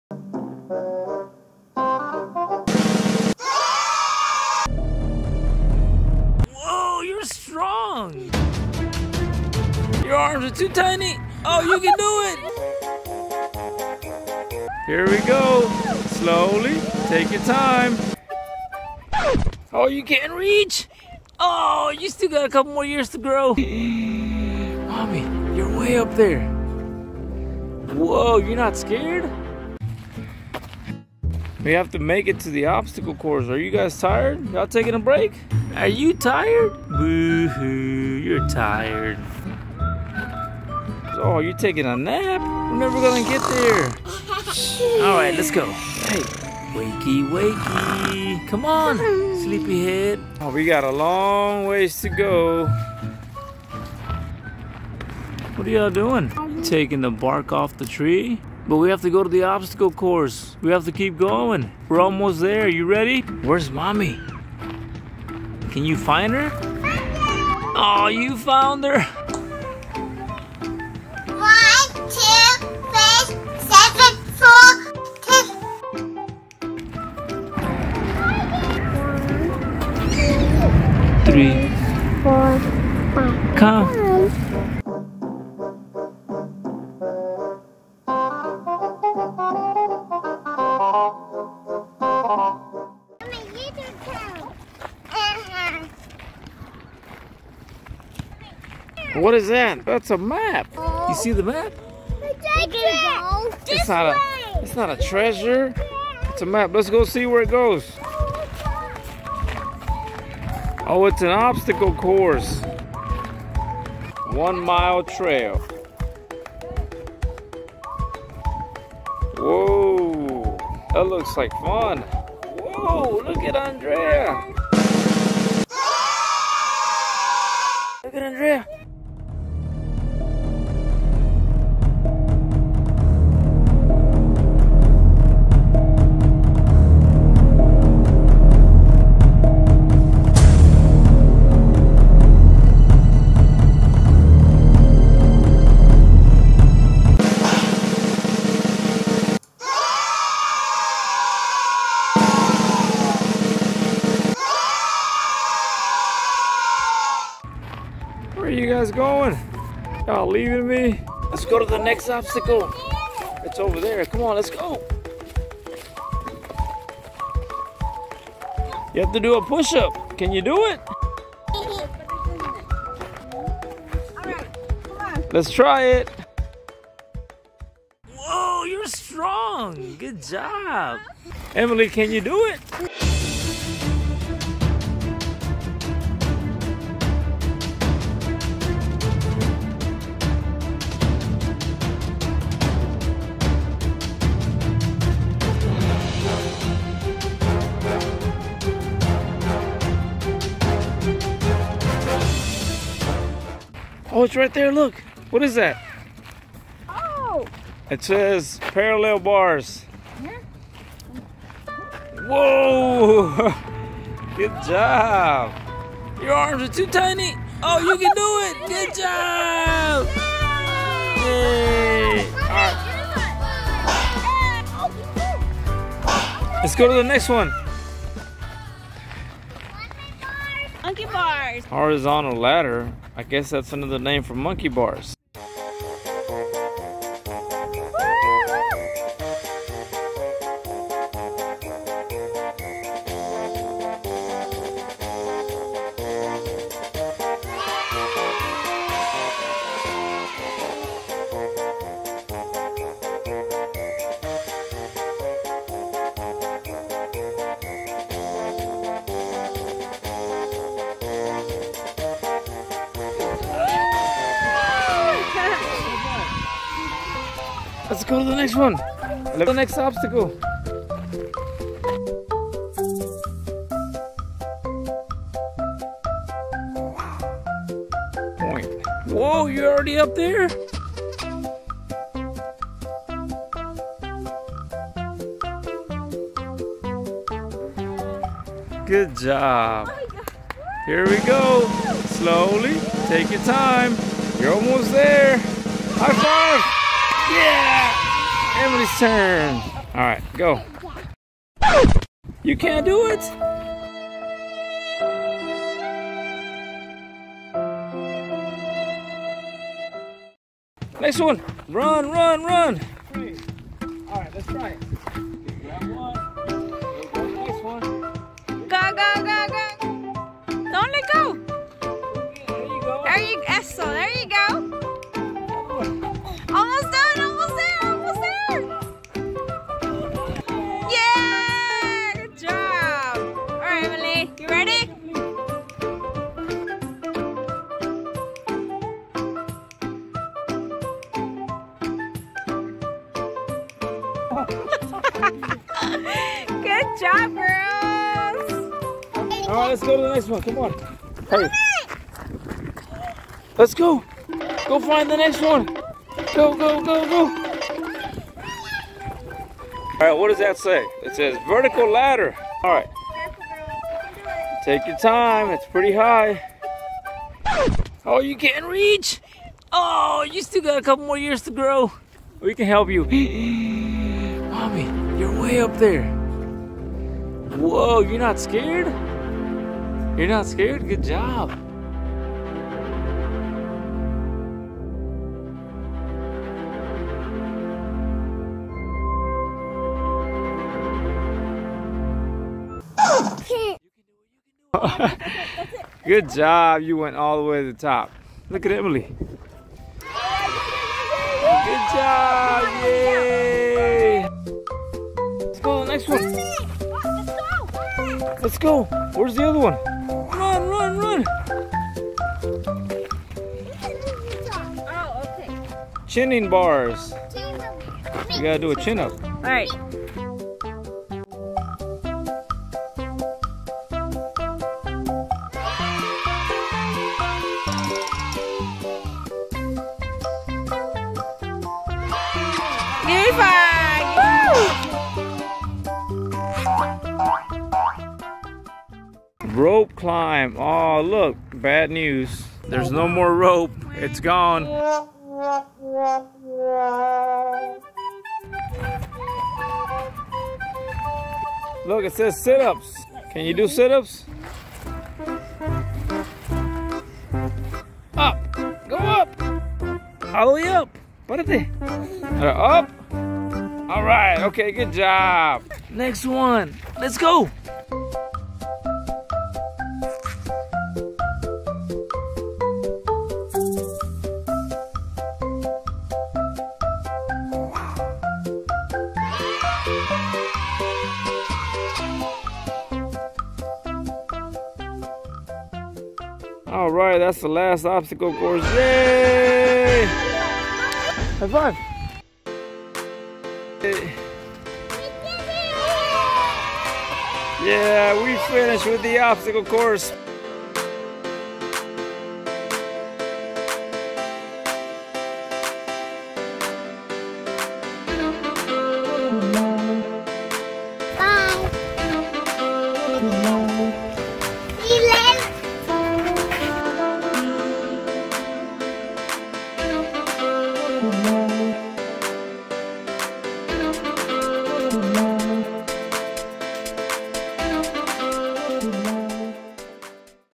Sisters challenge | Obstacle Course
Sisters challenge each other. Obstacle course. Watch us go through this obstacle course and have lots of fun. We do push-ups, sit-ups, monkey bars and much more. We walk a long and dangerous beam with fire. We get a little tired so we decide to take a little nap. Watch us have fun with our mom as we play hide and seek. Emily finds a treasure. We take a trip down a one mile trail filled with obstacles and adventures